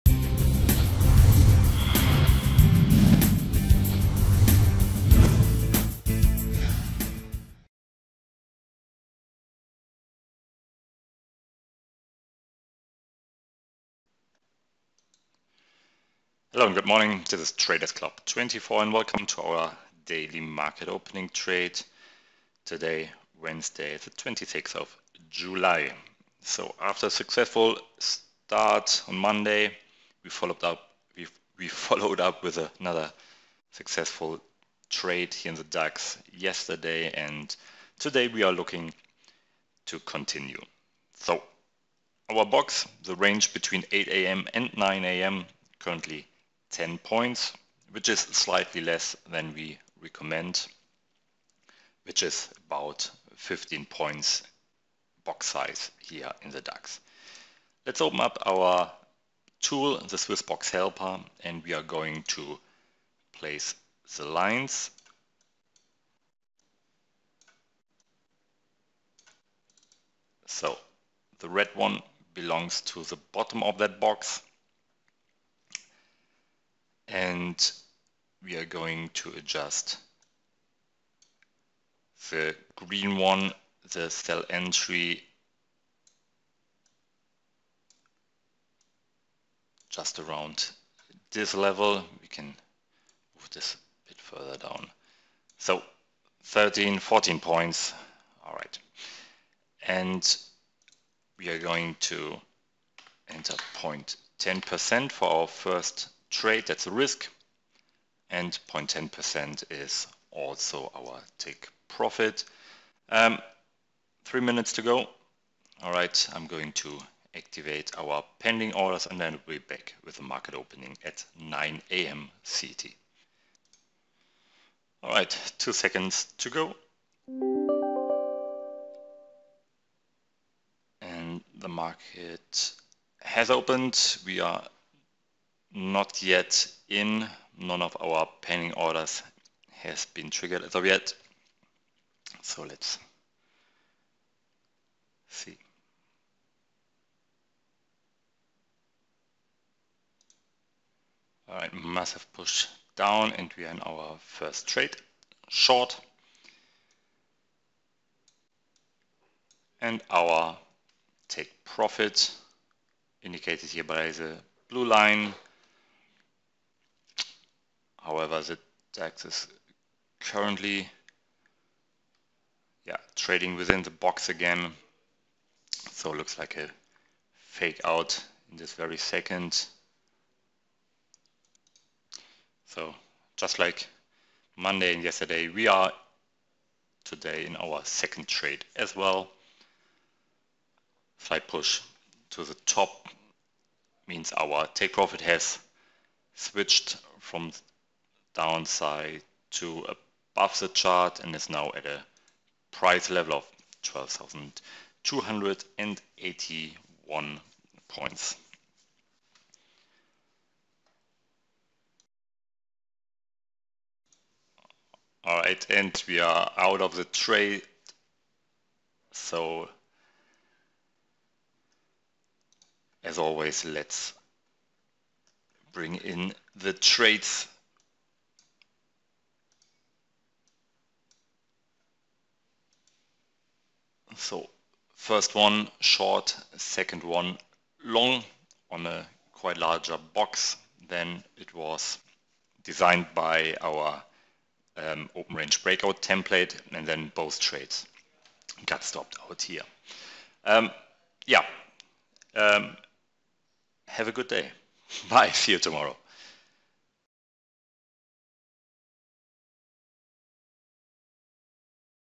TradersClub24 SwissBox OpenRange Breakout Live Trade Dax am July 26th 2017
TradersClub24 shows how to trade the DAX with the SwissBox open range breakout strategy. Get in touch with us for guest access or a demo trial